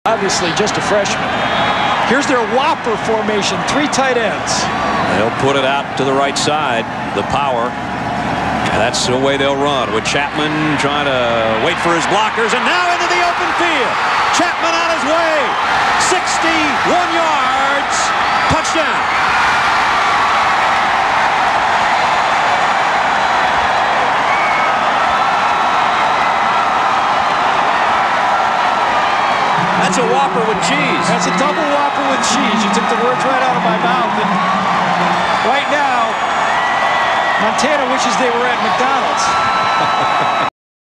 Marshall Thundering Herd - Doug Chapman TD run
Doug Chapman TD run vs Montana in 1-AA National title game.